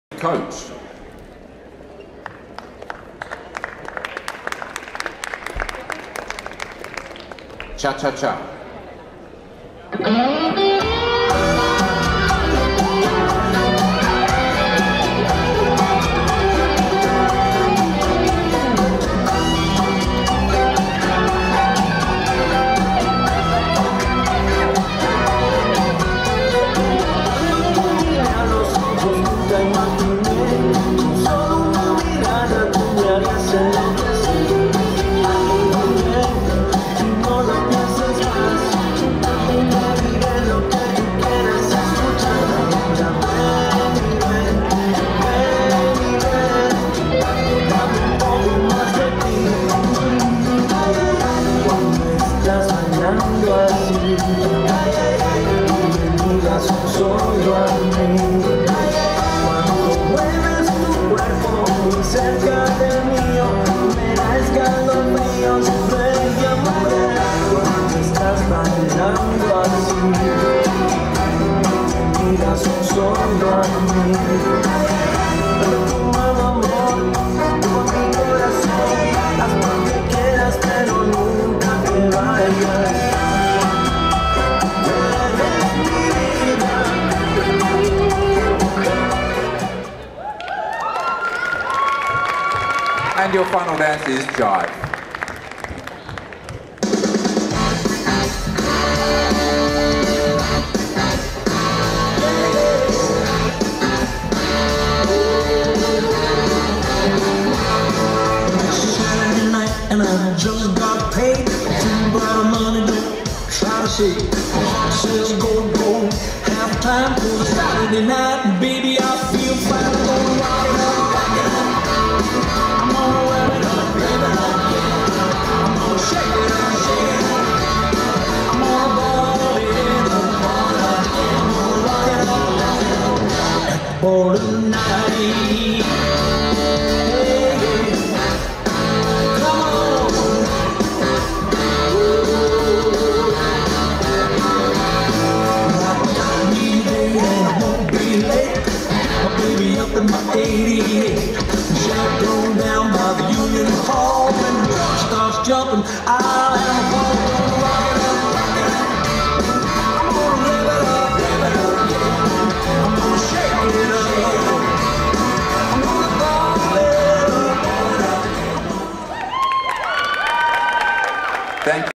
25 Coach Recreational Masters 1 Latin Final
Event from the WA Open DanceSport Competition held at Challenge stadium 24/10/2020